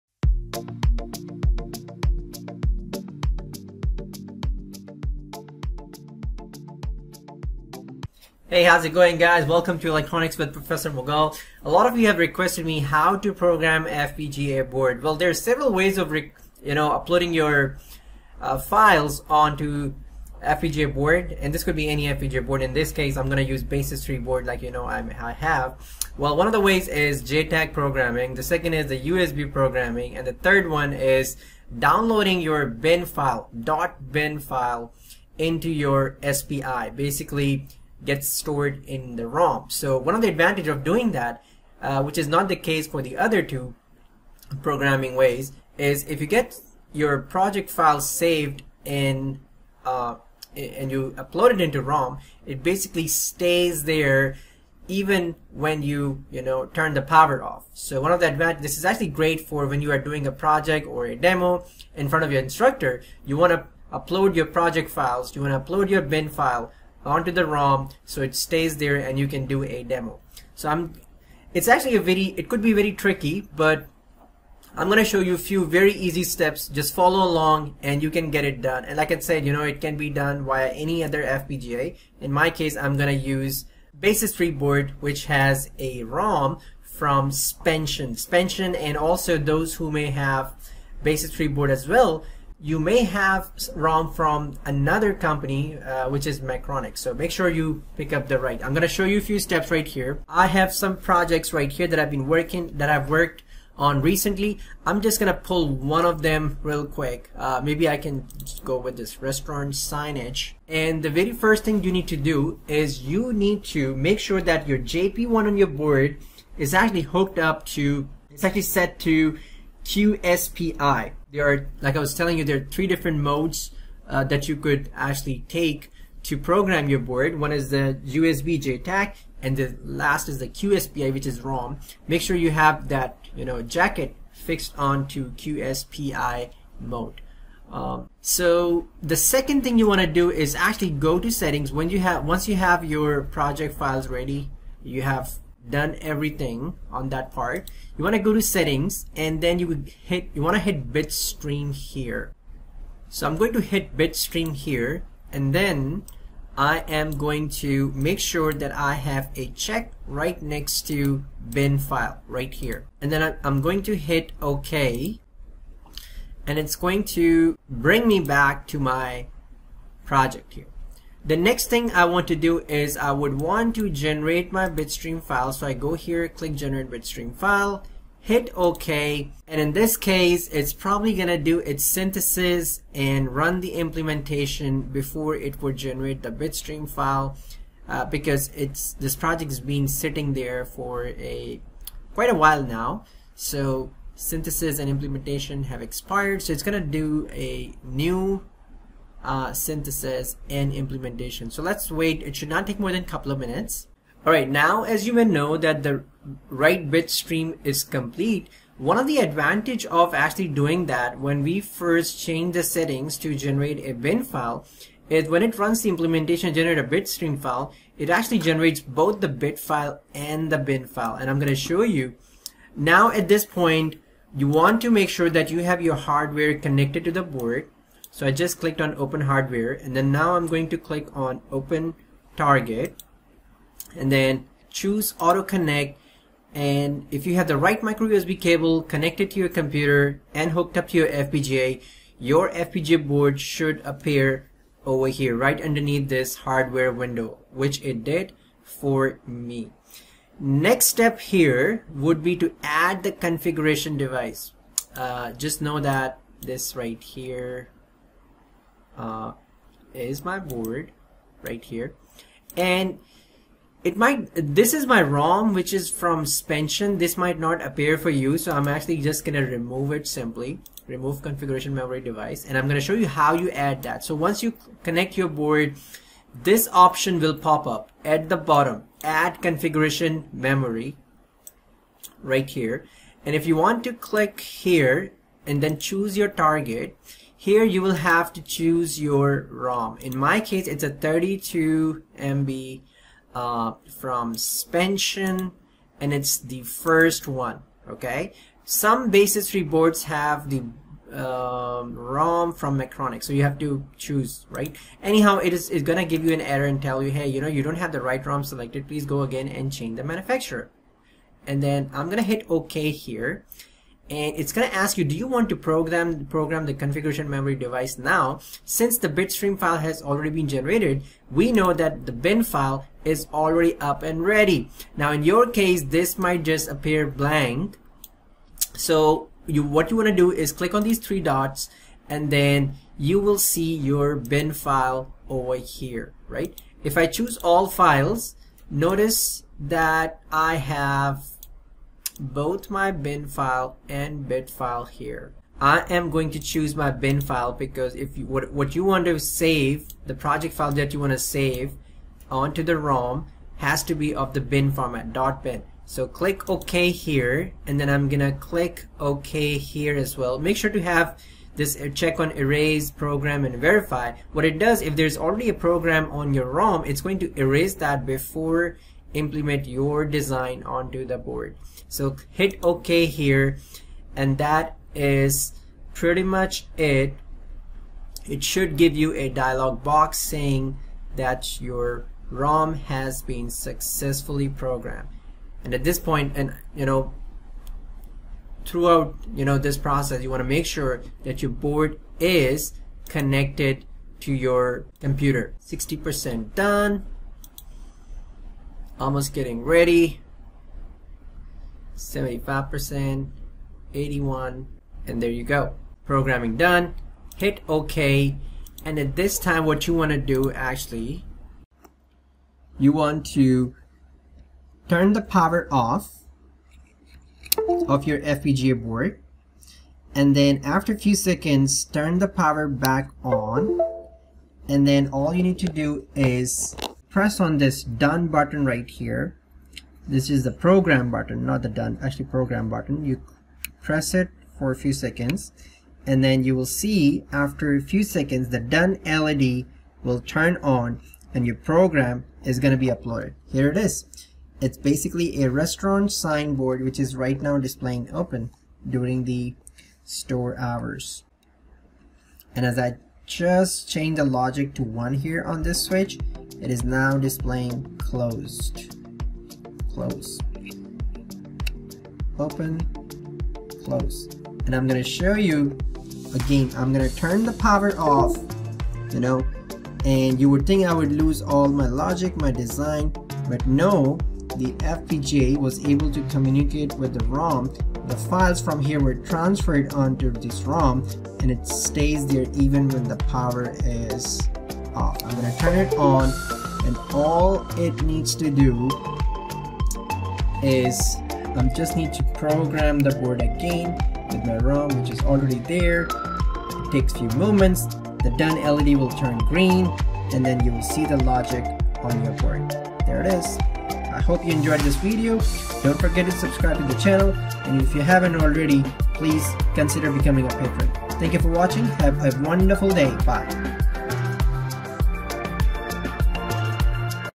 Programming FPGA Board using QSPI Protocol | Basys 3 Board
Learn how to program the FPGA board using QSPI protocol in few easy steps.

The Basys3 board contains a 32Mbit non-volatile serial Flash device, which is attached to the Artix 7 FPGA using a dedicated quad-mode (x4) SPI bus. FPGA configuration files can be written to the Quad SPI Flash (Spansion part number S25FL032), and mode settings are available to cause the FPGA to automatically read a configuration from this device at power on. An Artix-7 35T configuration file requires just over two Mbytes of memory, leaving approximately 48% of the flash device available for user data.

Credit: 👍
        -  Sound 🎵 : Camtesia